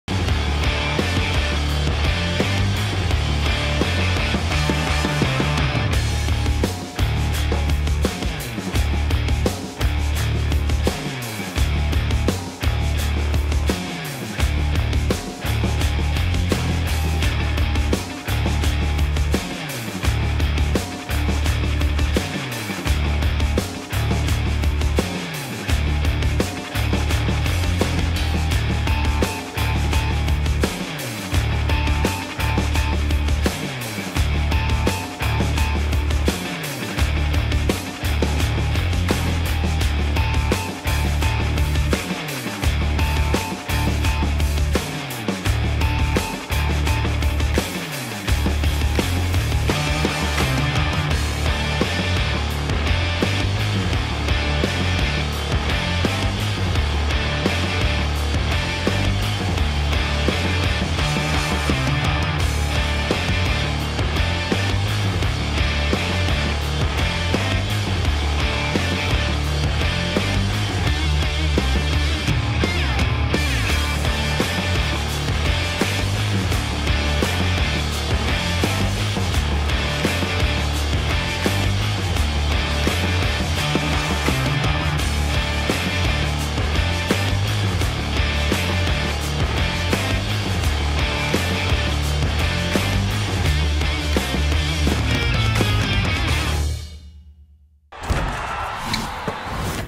Henk Norel deja el baloncesto
Clase, juego de pies, garra, carácter... Todo eso ha sido Henk Norel, que ha anunciado su retirada. Desde aquí queremos recordar su carrera en la Liga Endesa. ¡Qué talento!